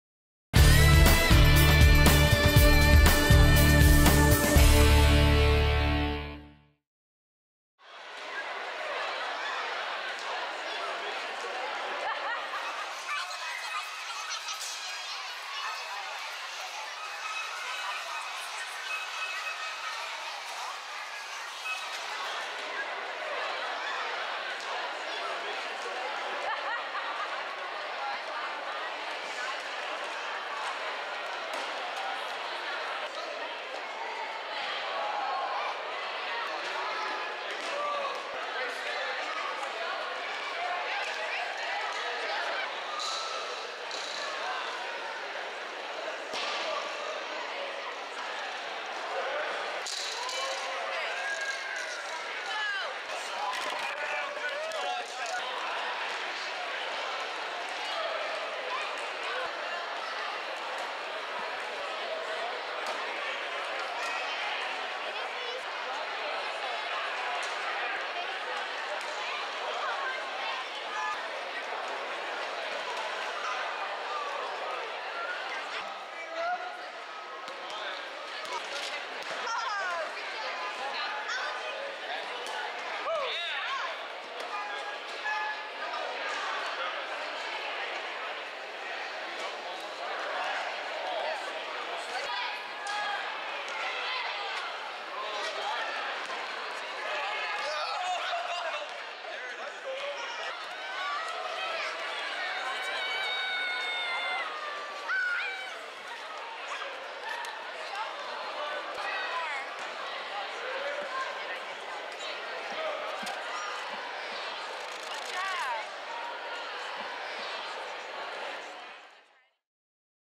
Tournament of Champions at McDaniel College
The Carroll County Therapeutic Recreation Council and McDaniel College co-sponsor the 22nd Annual Tournament of Champions held in the Gill Gymnasium on the campus of McDaniel College on April 7th, 2011. This event is designed for students with orthopedic, visual, and behavioral needs who participate in regular education classes.